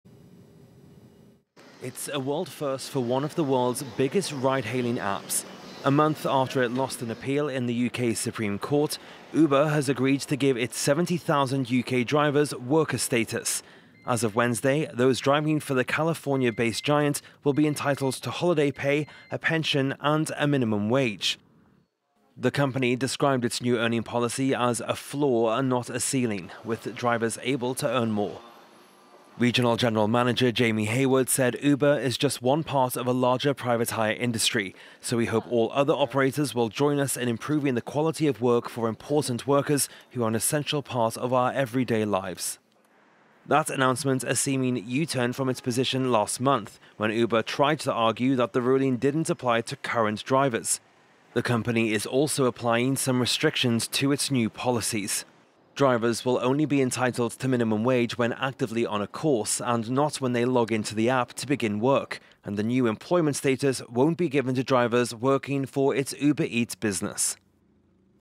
Uber grants UK drivers minimum wage, holiday pay and pensions in world first
Uber on Tuesday said it is granting its UK drivers worker status, with benefits including a minimum wage -- a world first for the US ride-hailing giant.